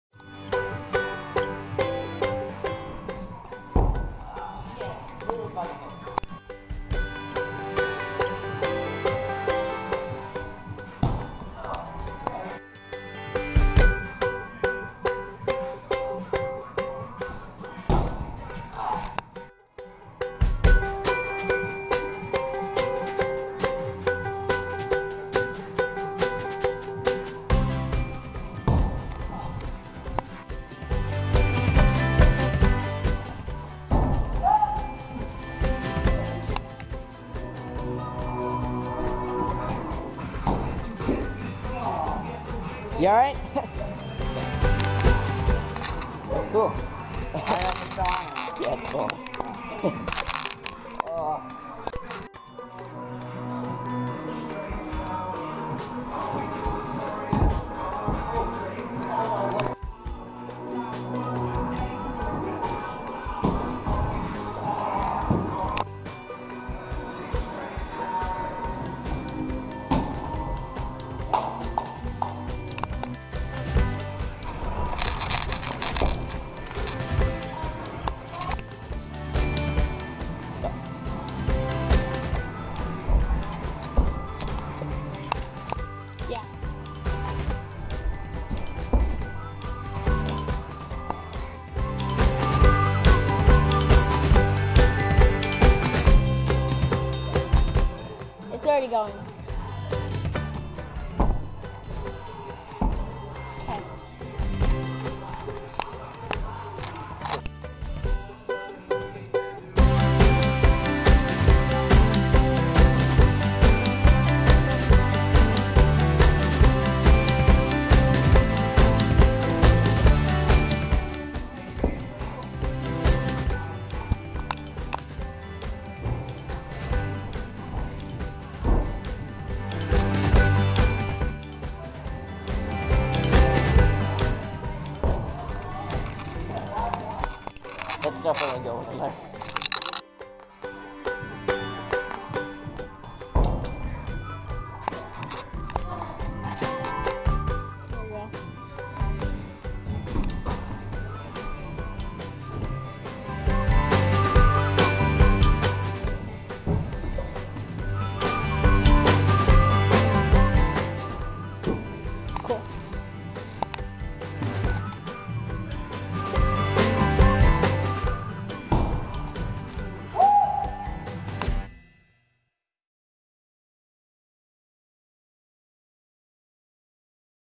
Parkour & Freerunning At IMA
Compilation of tricks we worked hard to get at the Infinite Movement Academy!